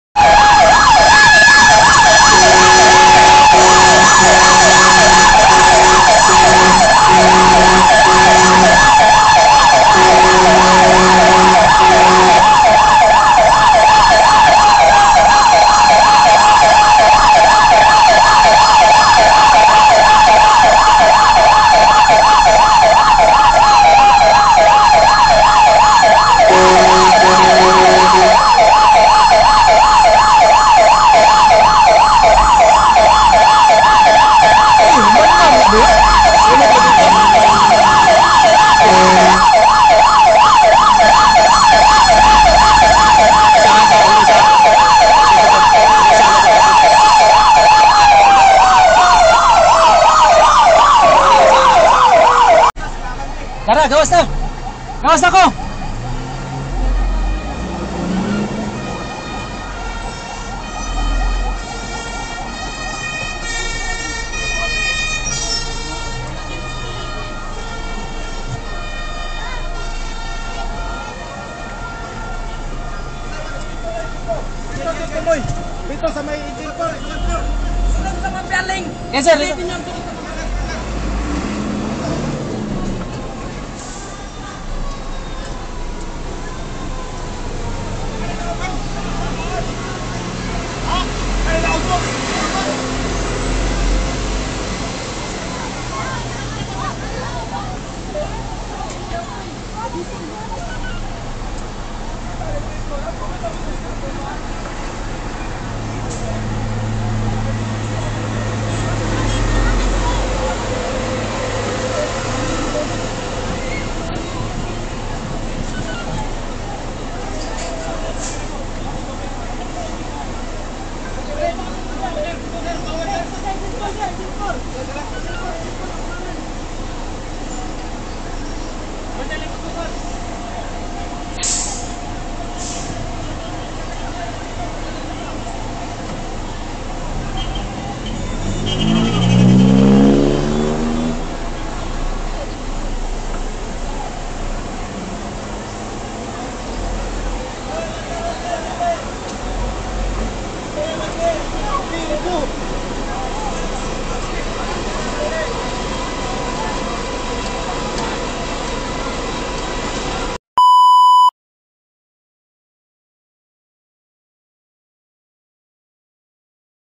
Responding 10-70 alarm at Mambaling, Cebu City 4/19/23 (watch til the end)
The copyright of this video belongs to @RVKatana
please refrain from unauthorized editing and redistribution

FTO diaries - 10-70 Alarm at Sitio Mangga, Brgy Mambaling, Cebu City - April 19, 2023 time of incident 1346H

FTOxPOV